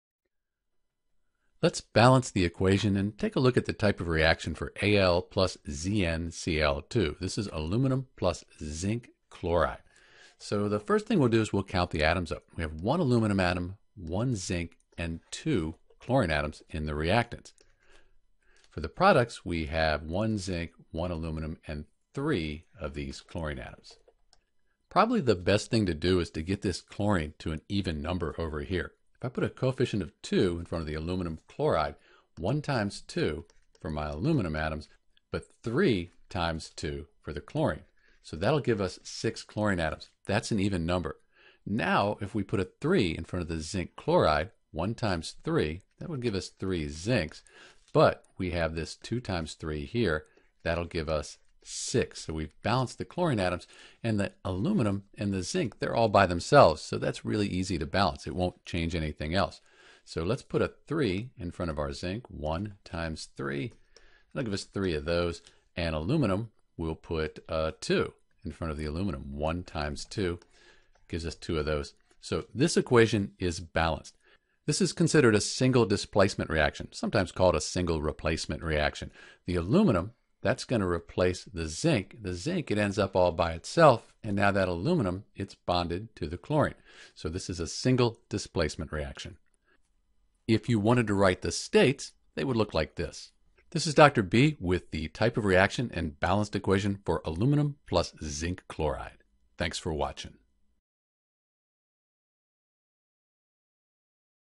How to Balance Al + ZnCl2 = Zn + AlCl3 (and Type of Reaction)
In this video we'll balance the equation Al + ZnCl2 = Zn + AlCl3 and provide the correct coefficients for each compound.

To balance Al + ZnCl2 = Zn + AlCl3 you'll need to be sure to count all of atoms on each side of the chemical equation.

Once you know how many of each type of atom you can only change the coefficients (the numbers in front of atoms or compounds) to balance the equation for Aluminum + Zinc Chloride.

Important tips for balancing chemical equations:
dogs!!!